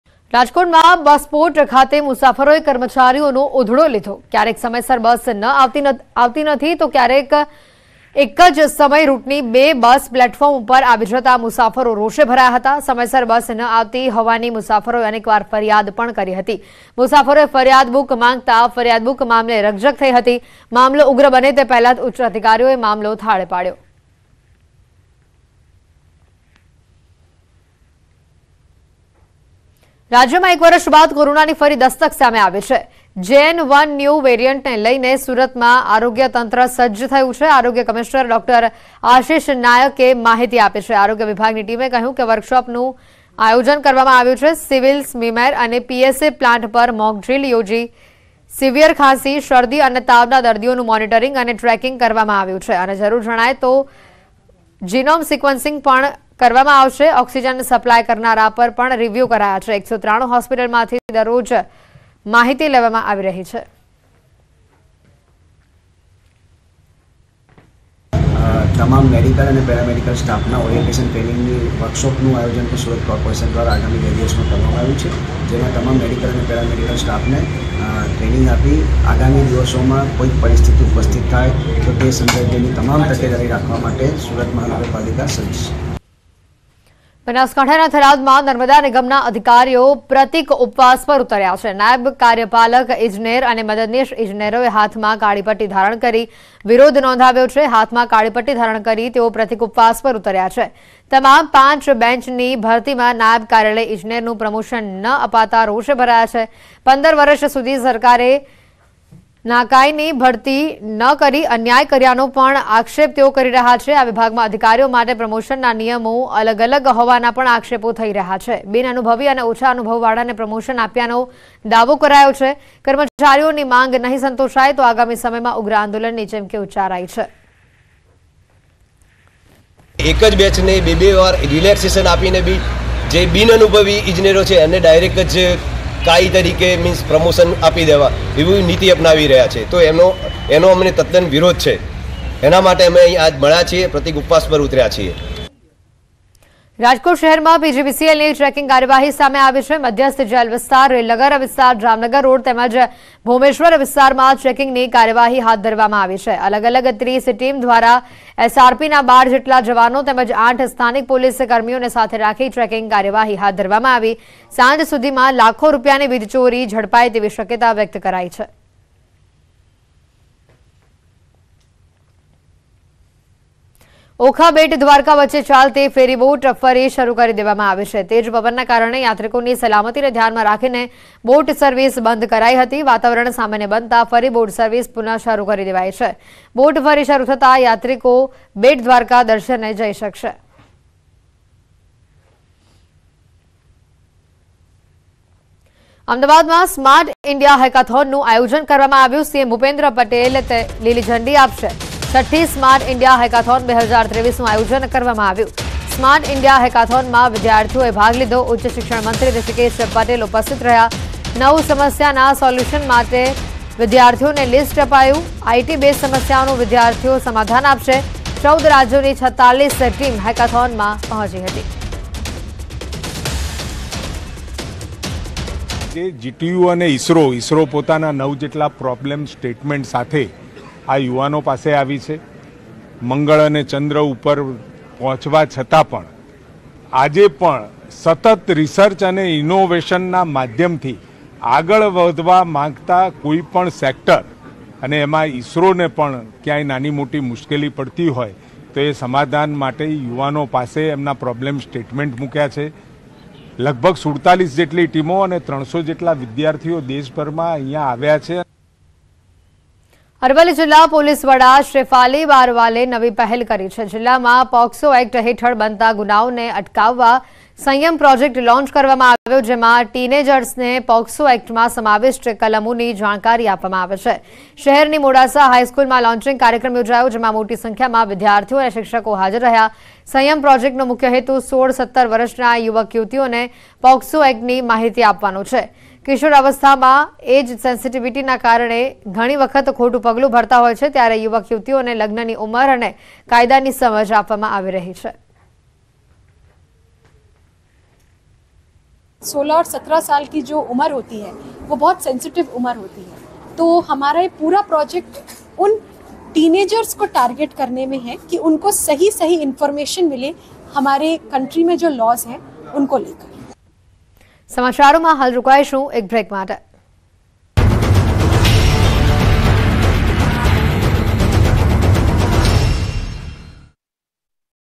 Rajkot News | સમયસર બસ ન આવતા અટવાયા મુસાફરો | Passengers | Gujarat News
Passengers stranded in Rajkot as buses do not arrive on time

રાજકોટમાં સમયસર બસ ન આવતા અટવાયા મુસાફરોn18oc_Rajkot